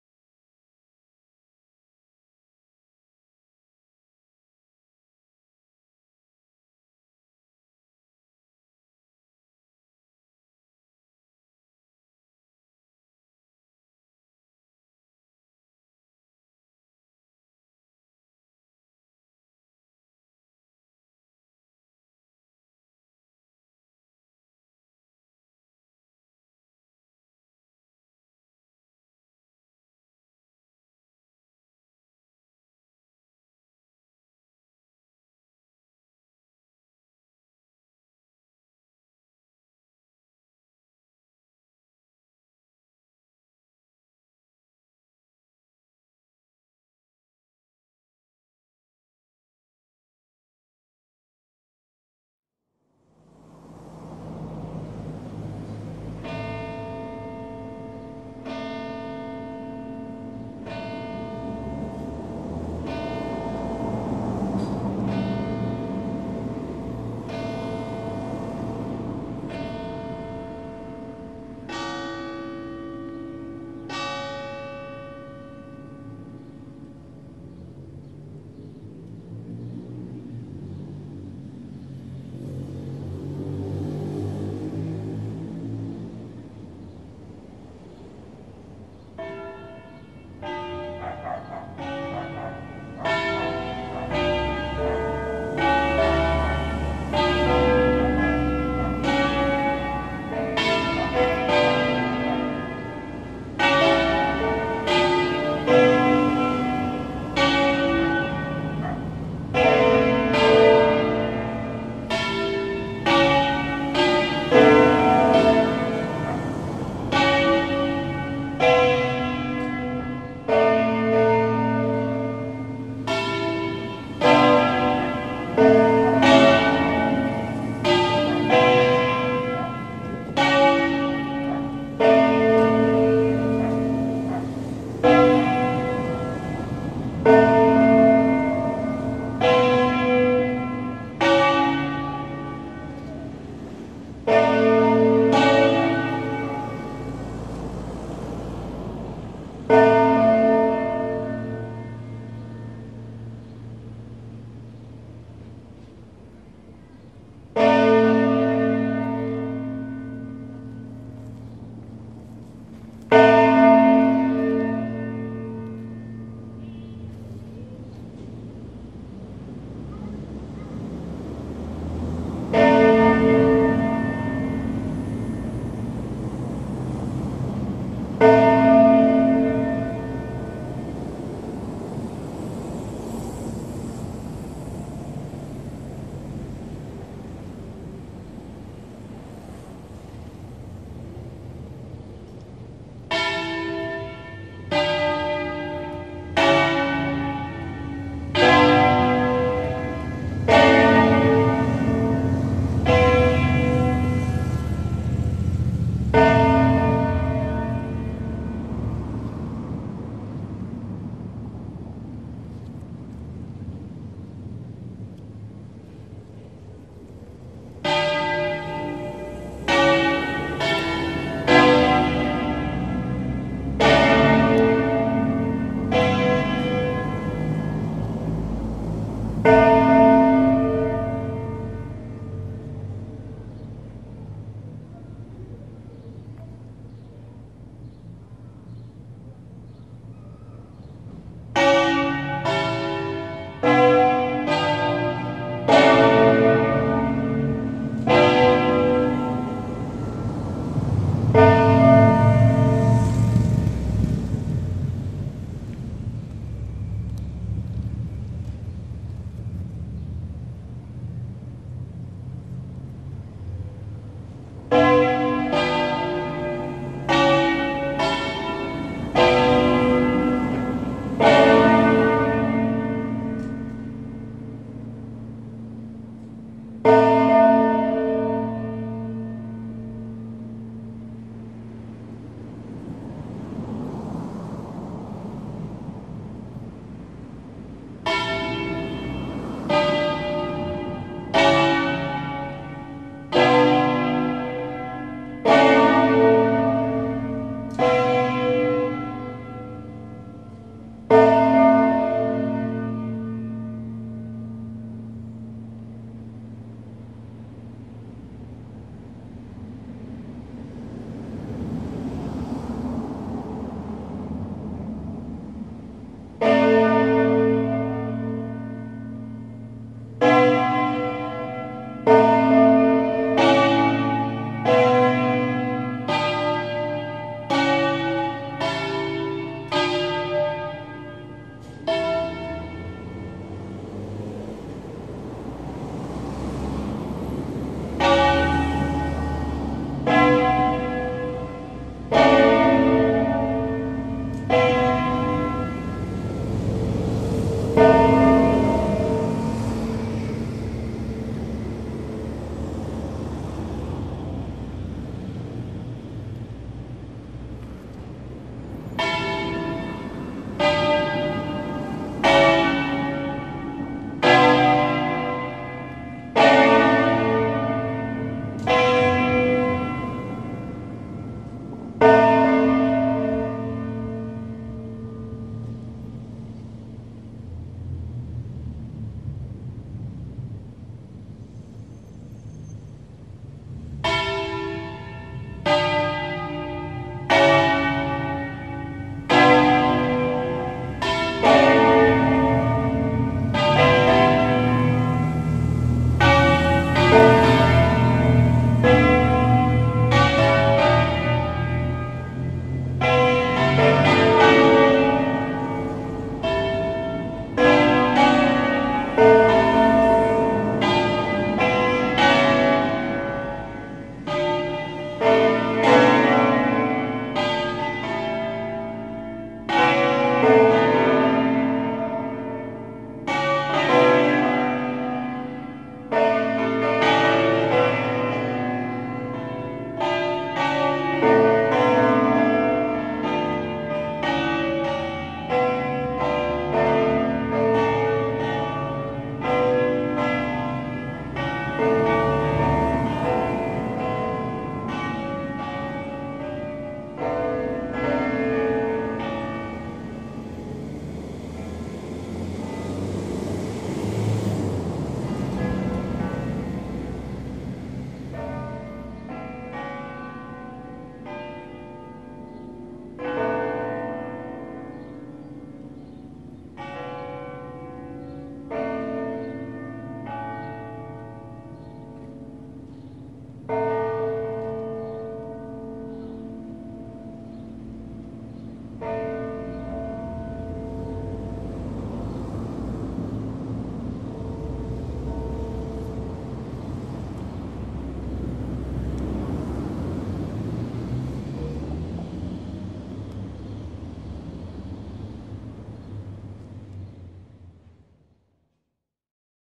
Le campane di Nembro (BG) - Concerto delle 5 grosse
Concerto di 8 campane in La2 cresc.
Angelo Ottolina 1952
Concerto solenne delle 5 campane maggiori per l'Angelus serale in occasione della novena per la Madonna dello Zuccarello. Il concerto è stato memorizzato con la funzione di acquisizione dei tempi, che sono stati ricalibrati in seguito alla manutenzione. Audio HD registrato con Zoom H2